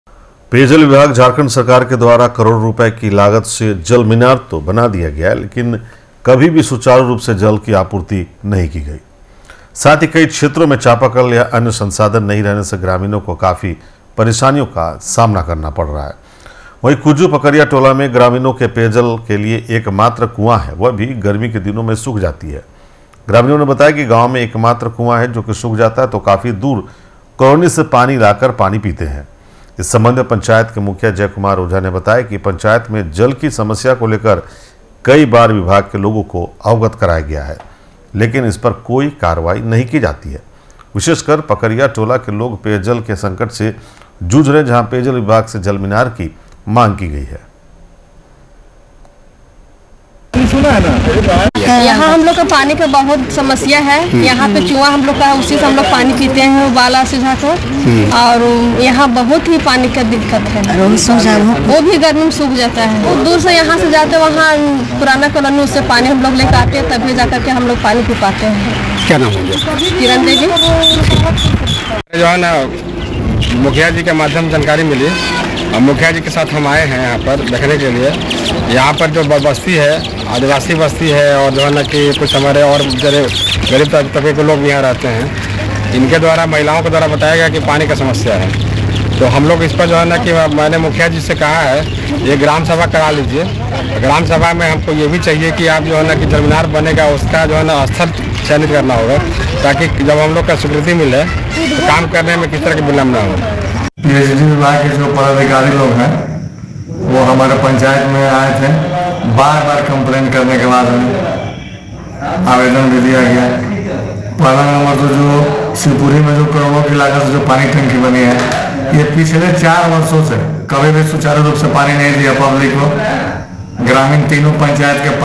कुजू /रामगढ़ : पेयजल के समस्या को लेकर कार्यवाही, जांच करने पहुंचे पेयजल विभाग के पदाधिकारीगण
Raftaar Media